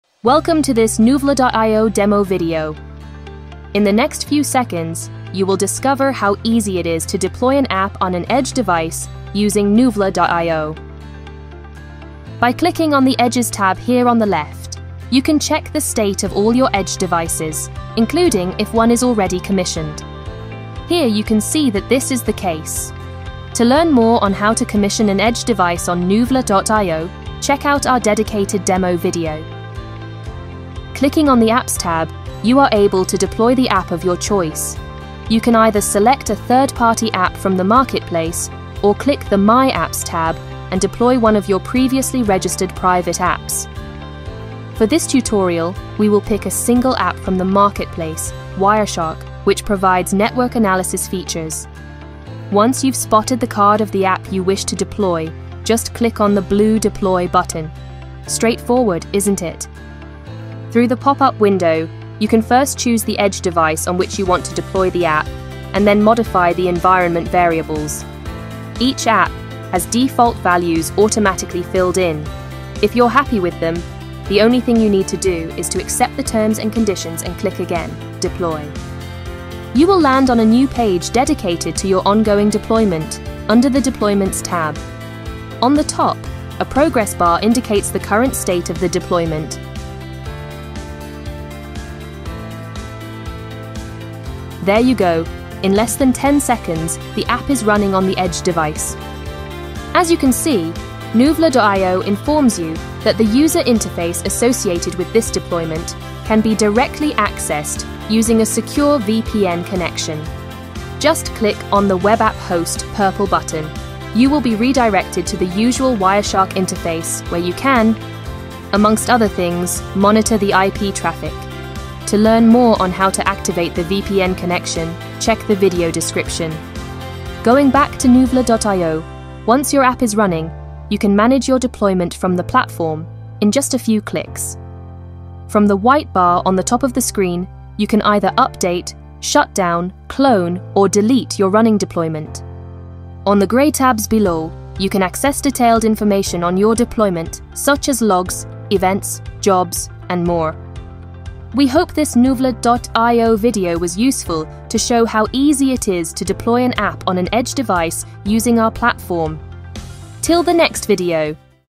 Find out how easy it is to deploy an app on an edge computing device with Nuvla.io
Nuvla.io is a B2B SaaS edge-to-cloud management platform which accelerates the deployment of apps at the edge, reducing risk, removing management complexity and delivering full control over data.

This video will you will discover how easy it is to deploy an app on an edge device using Nuvla.io. If you have any questions or need further assistance, don't hesitate to get in touch.

Here's the script:

Welcome to this Nuvla.io demo video!
In the next few seconds, you will discover how easy it is to deploy an app on an edge device using Nuvla.io.

By clicking on the Edges tab here on the left, you can check the state of all your edge devices, including if one is already commissioned. Here you can see that this is the case. To learn more on how to commission an edge device on Nuvla.io, check out our dedicated demo video.

Clicking on the Apps tab, you are able to deploy the app of your choice. You can either select a third-party app from the Marketplace or click the “My Apps” tab and deploy one of your previously registered private apps.

For this tutorial, we will pick a single app from the Marketplace, Wireshark, which provides network analysis features.

Once you’ve spotted the card of the app you wish to deploy, just click on the blue “Deploy” button. Straightforward, isn’t it?

Through the pop-up window, you can first choose the edge device on which you want to deploy the app and then modify the environment variables.

You will land on a new page dedicated to your ongoing deployment, under the Deployments tab. On the top, a progress bar indicates the current state of the deployment.

There you go! In less than 10 seconds, the app is running on the edge device.

As you can see, Nuvla.io informs you that the user interface associated with this deployment can be directly accessed using a secure VPN connection. Just click on the “webapp host” purple button: you will be redirected to the usual Wireshark interface where you can, amongst other things, monitor the IP traffic. To learn more on how to activate the VPN connection, check the video description.
Going back to Nuvla.io, once your app is running, you can manage your deployment from the platform, in just a few clicks. From the white bar on the top of the screen, you can either update, shutdown, clone or delete your running deployment. On the grey tabs below, you can access detailed information on your deployment, such as logs, events, jobs, and more.
We hope this Nuvla.io video was useful to show how easy it is to deploy an app on an edge device using our platform.

Till the next video!